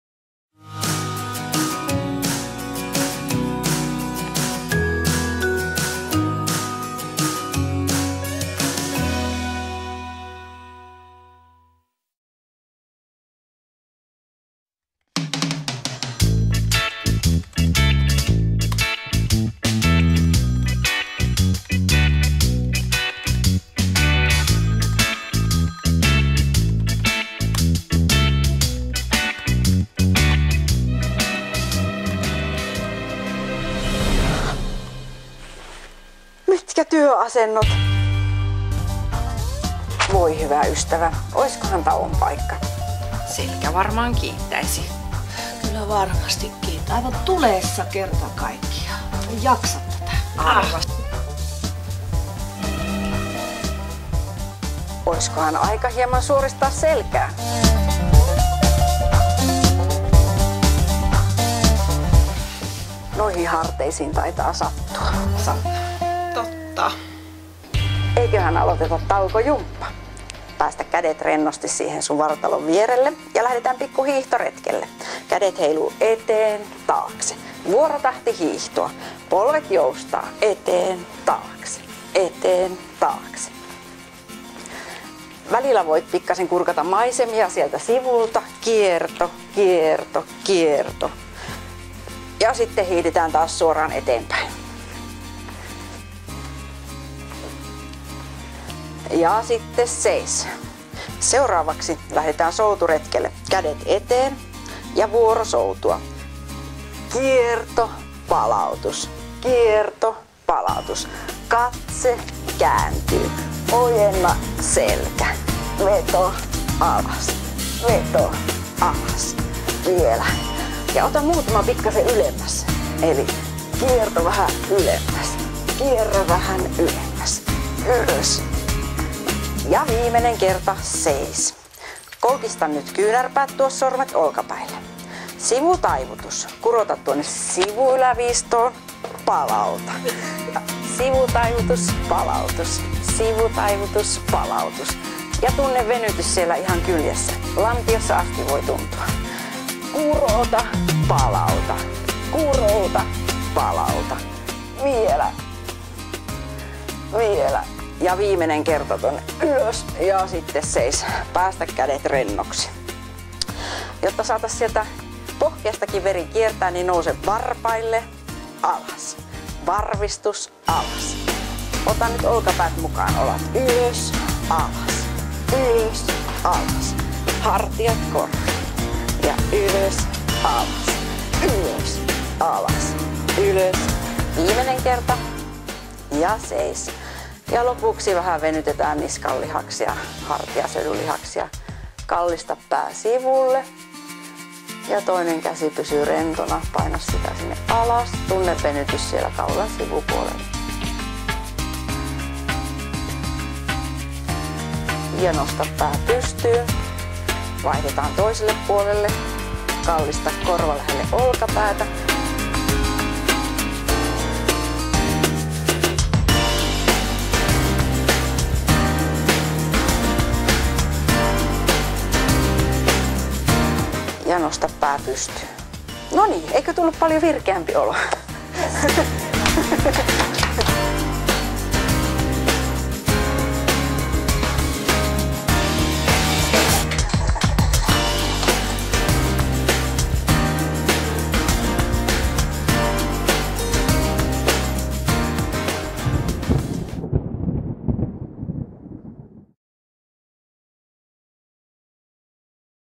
Liikkuva Liminka - Taukojumppa
Asiantuntijoiden suunnittelema helppo ja piristävä taukojumppa. Lyhyenkin liikkumisen aikana elimistön energianaineenvaihdunnan vilkastuu, veren virtaus lisääntyy ja mieli virkistyy. Liikkeiden suunnittelusta on vastannut ODL Liikuntaklinikka.